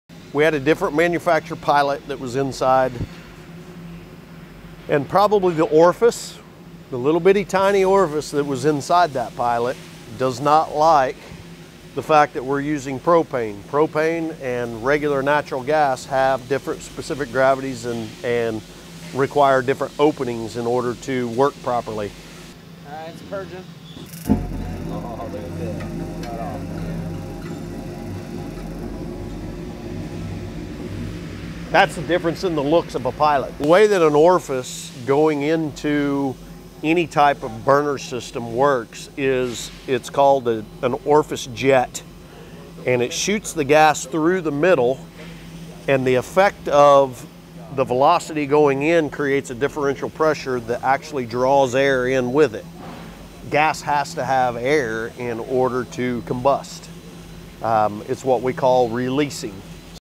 Orifice Sizing and Air-Gas Balance of Gas Burners
From orifice sizing to the perfect air-gas balance, join us as we explore the intricacies of propane and natural gas burners. 🔥Discover the key factors that ensure a flawless ignition for optimal performance.